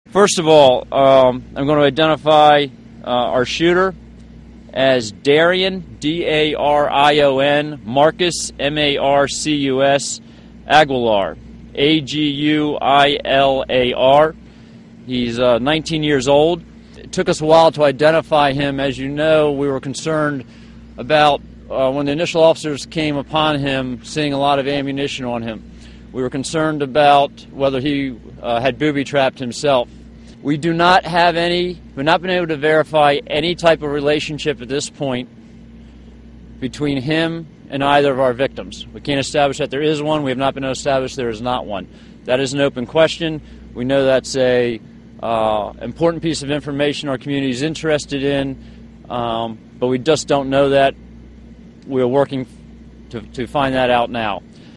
Police Identify Shooter in Md. Mall Shooting
Police have identified the shooter in the Maryland mall shooting as 19-year-old Darion Marcus Aguilar from College Park. Police say he shot to death two people at a store on the upper level before killing himself on Saturday. (Jan. 26)